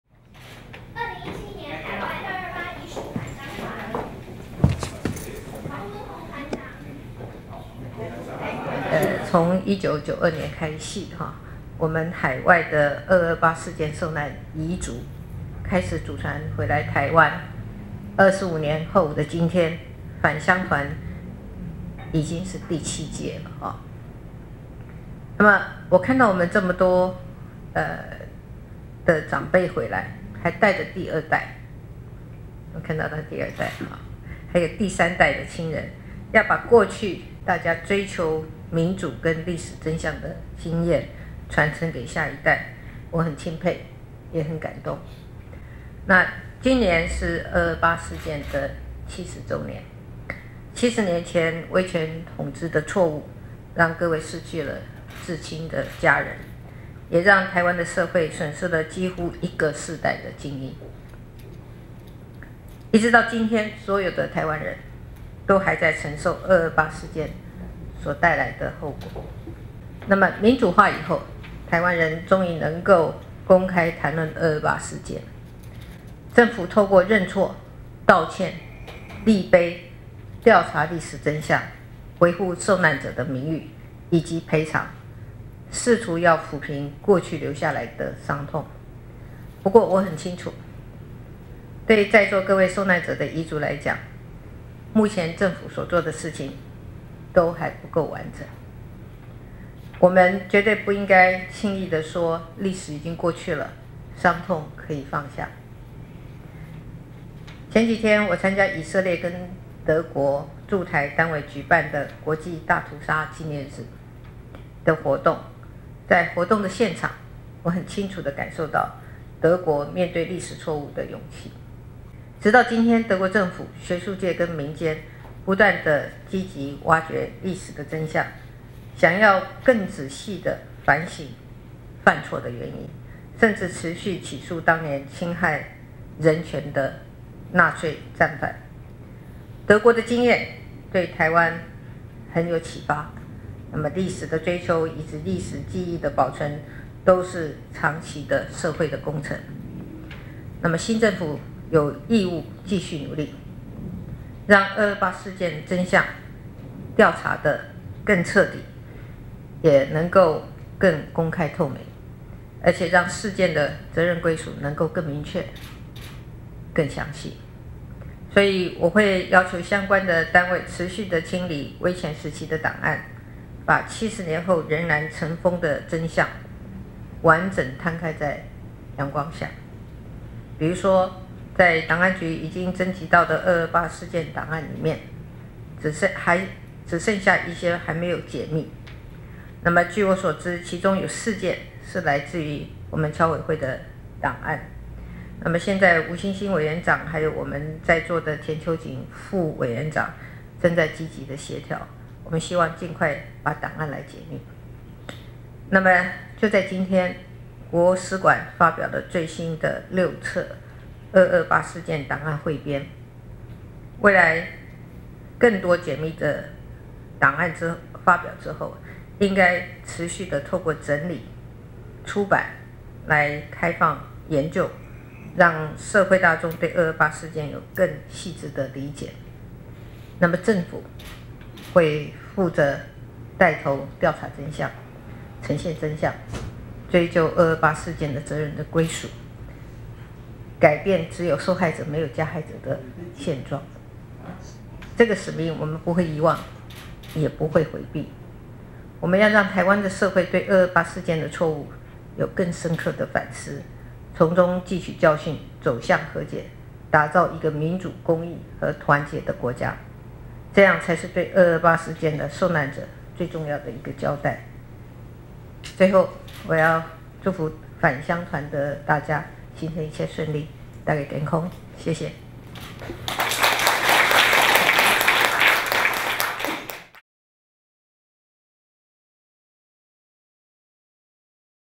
蔡英文：政府會帶頭調查追究二二八責任歸屬
蔡英文總統今天（2/23）上午接見「2017年海外二二八遺屬返鄉團」，強調政府會帶頭調查真相、呈現真相，追究二二八事件的責任歸屬，以改變「只有受害者、沒有加害者」的現狀，讓台灣社會記取教訓，走向和解，打造一個民主、公義和團結的國家。

蔡英文致詞時表示，1992年，海外的二二八事件受難者遺屬開始組團回來台灣。25年後的今天，返鄉團已經是第7屆了。看到有這麼多長輩回來，還帶著第二代、第三代的親人，要把過去大家追求民主和歷史真相的經驗，傳承給下一代，她很欽佩，也很感動。

蔡英文說，今（106）年是二二八事件70週年。70年前，威權統治的錯誤，讓各位失去至親家人，也讓台灣社會損失了幾乎一整個世代的菁英。一直到今天，所有的台灣人都還在承受二二八事件所帶來的後果。

蔡英文表示，民主化以後，台灣人終於能夠公開談論二二八事件。政府透過認錯、道歉、立碑、調查歷史真相、回復受難者名譽，以及賠償，試圖要撫平過去留下來的傷痛。她很清楚，對在座各位受難者的遺屬來說，目前政府所做的事情都還不夠完整。我們絕對不應該輕易地說，歷史已經過去了，傷痛可以放下。

蔡英文提到，前幾天參加以色列和德國駐台單位舉辦的「國際大屠殺紀念日」活動。在活動現場，清楚感受到德國面對歷史錯誤的勇氣。直到今天，德國的政府、學術界和民間，不斷積極挖掘歷史真相，想要更仔細地反省犯錯的原因，甚至持續起訴當年侵害人權的納粹戰犯。德國的經驗對台灣很有啟發。歷史真相的追求，以及歷史記憶的保存，都是長期的社會工程。

蔡英文強調，新政府有義務繼續努力，讓二二八事件的真相，調查得更徹底，也更公開透明，並且讓事件的責任歸屬，能夠更明確、更詳細。所以，她會要求相關單位，持續清理威權時期的檔案，把70年後仍然塵封的真相，完整攤開在陽光下。

蔡英文舉例說，在檔案局已經徵集到的二二八事件檔案裡面，只剩下一些還沒有解密。據她所知，其中有4件，是來自僑委會的檔案。現在，吳新興委員長，還有在座的田秋堇副委員長，正在積極協調，希望儘快將檔案解密。

蔡英文說，就在今天，國史館發表了最新的6冊「二二八事件檔案彙編」。未來更多解密後的檔案發表之後，應該持續透過整理、出版，來開放研究，讓社會大眾對二二八事件，有更細緻的理解。

蔡英文表示，政府會帶頭調查真相、呈現真相，追究二二八事件的責任歸屬，改變「只有受害者、沒有加害者」的現狀。這個使命，我們不會遺忘，也不會迴避。我們要讓台灣社會，對二二八事件的錯誤有更深刻的反思，從中記取教訓，走向和解，打造一個民主、公義和團結的國家。這樣，才是對二二八事件的受難者，最重要的一個交代。